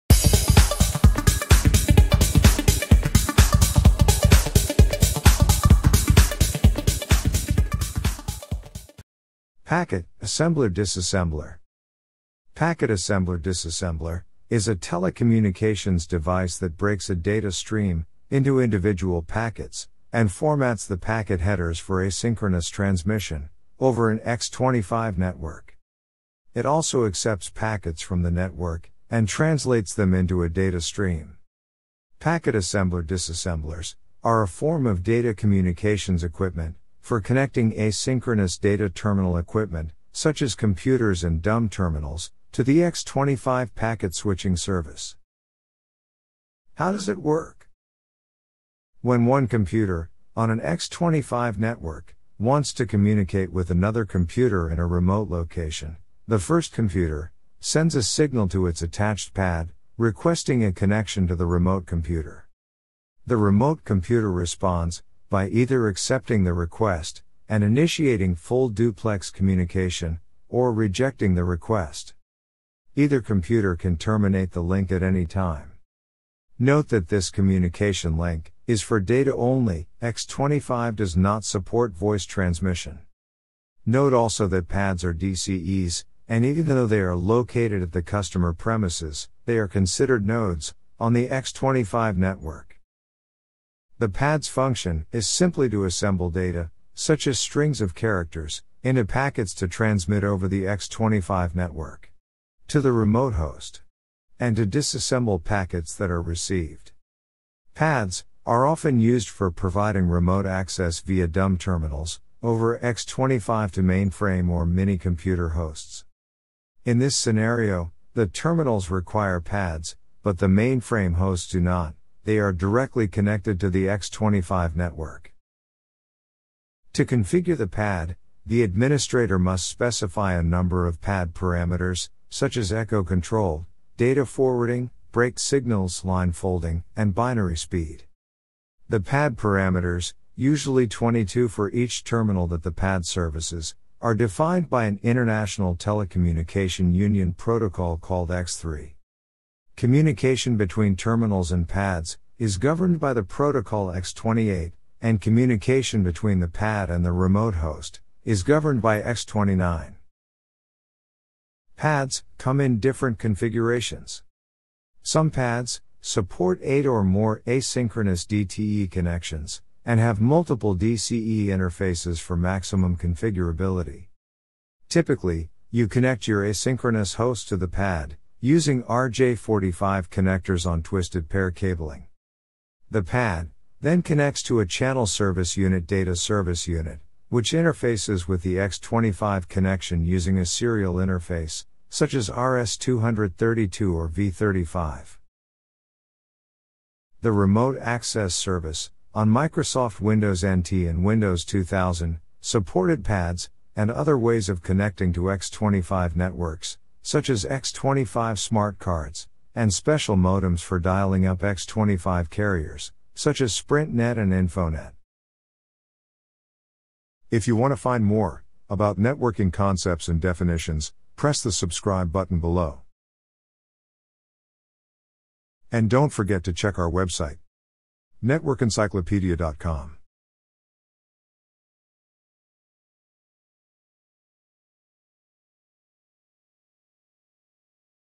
Packet Assembler/Disassembler (PAD) - Network Encyclopedia
Packet Assembler/Disassembler is a telecommunications device that breaks a data stream into individual packets and formats the packet headers for asynchronous transmission over an X.25 network.

It also accepts packets from the network and translates them into a data stream.

When one computer on an X.25 network wants to communicate with another computer in a remote location, the first computer sends a signal to its attached PAD requesting a connection to the remote computer.

The remote computer responds by either accepting the request and initiating full-duplex communication or rejecting the request.

Either computer can terminate the link at any time.